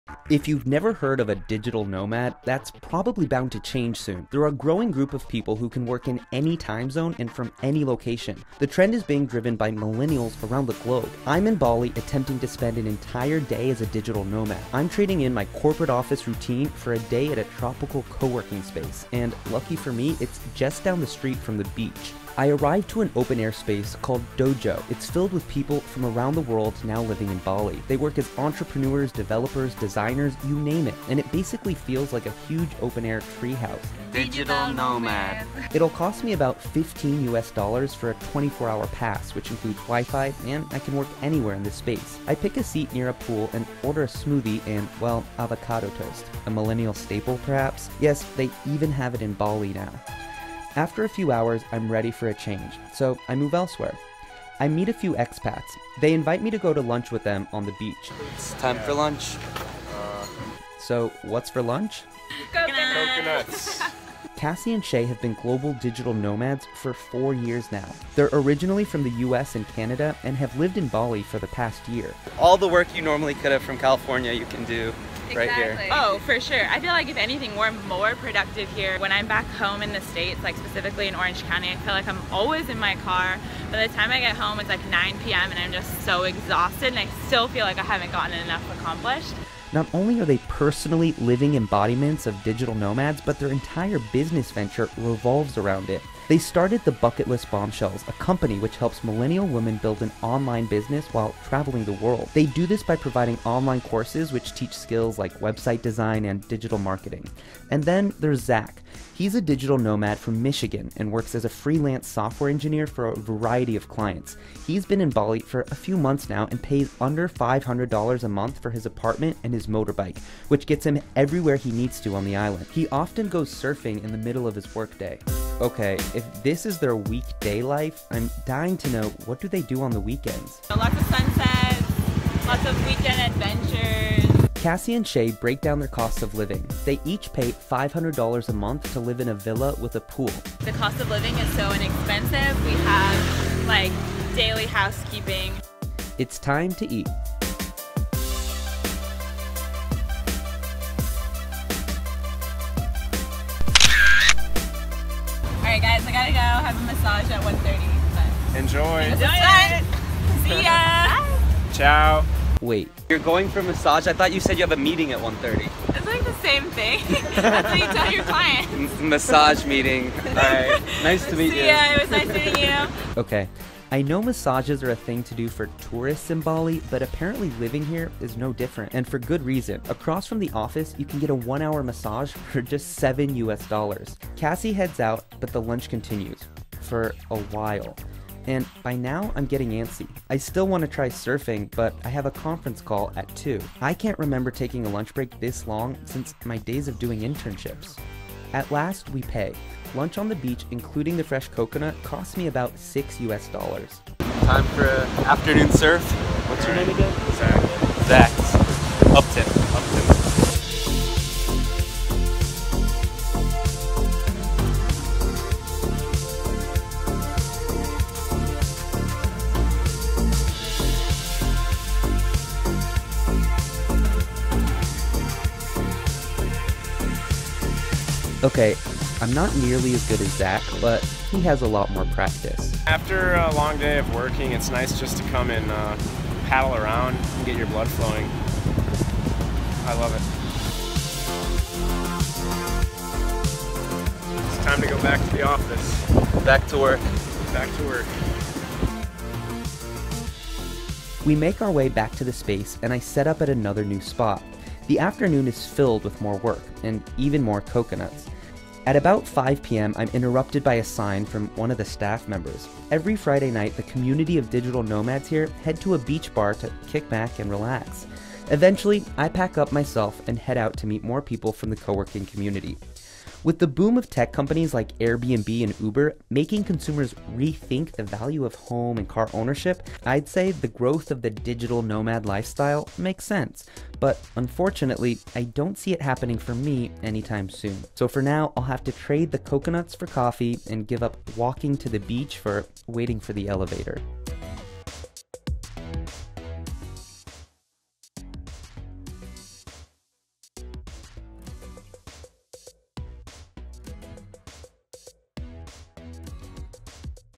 I tried living as a digital nomad for the day | CNBC Reports
Coconuts, surfing, and a massage ... all in a day's work? CNBC's Uptin Saiidi spends a day in Bali to test out a new workday routine and meet global digital nomads.

CNBC_international